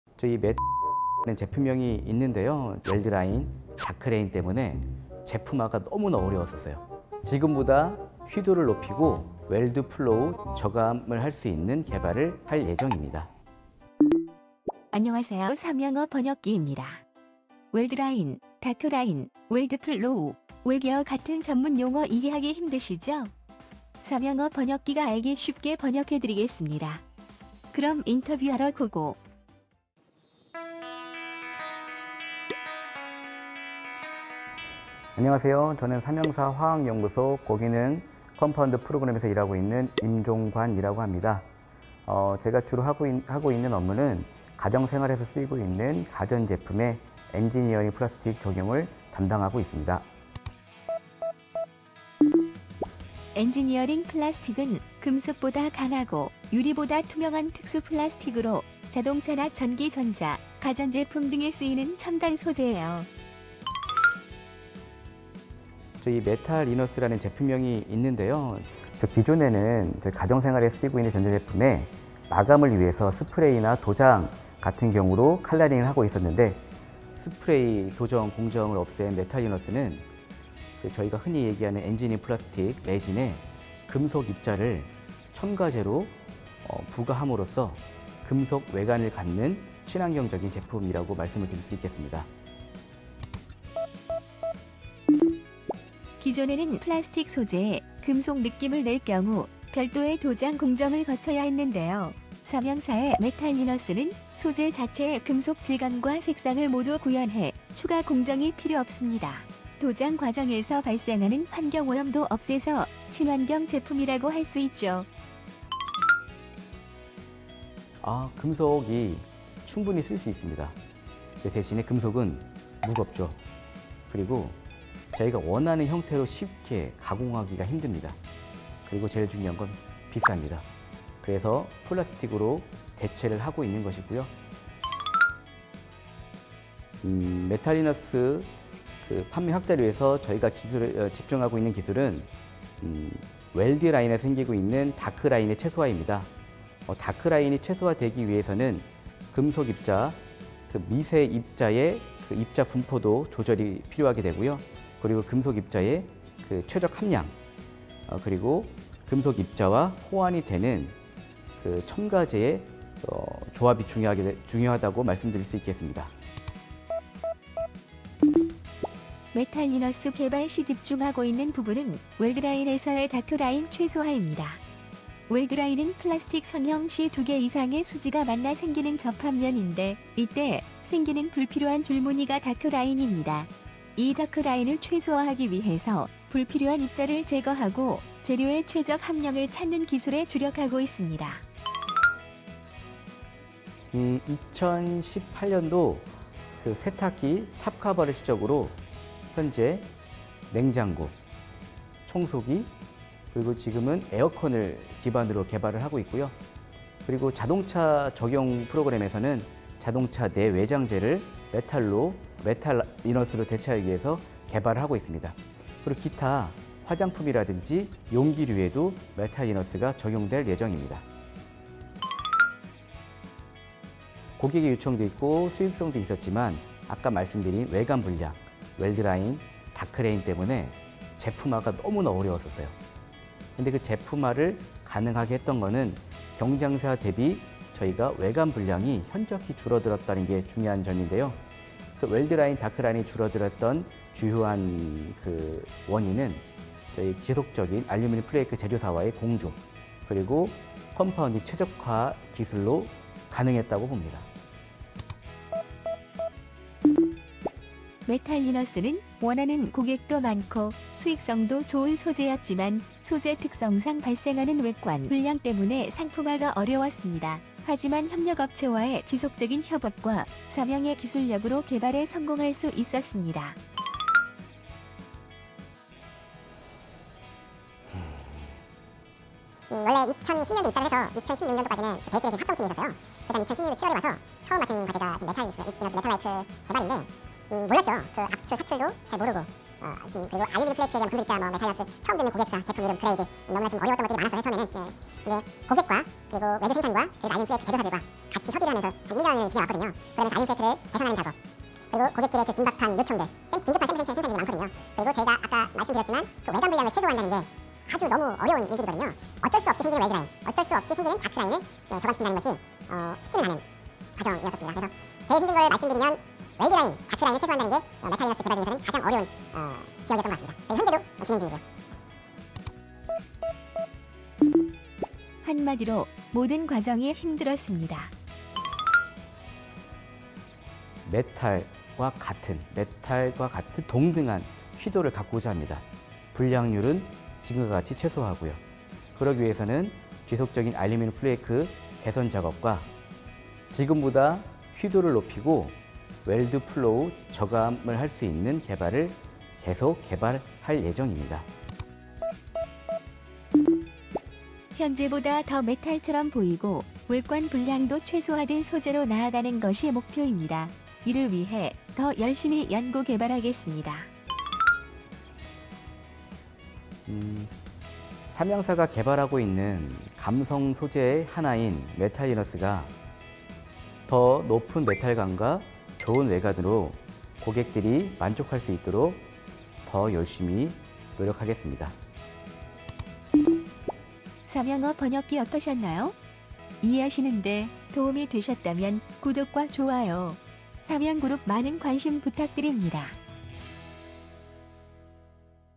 '삼양어 번역기'가 알려주는 첨단 화학 소재, 메탈리너스✨
평소에 이해하기 어려웠던 전문 용어들을
핵심만 딱 찝어 알기 쉽게 설명해준다!
삼양그룹 1타 강사 ‘삼양어 번역기’ 등장🙌

삼양그룹의 첨단 화학 소재와 기술력
삼양어 번역기와 함께라면 완전정복 가능👌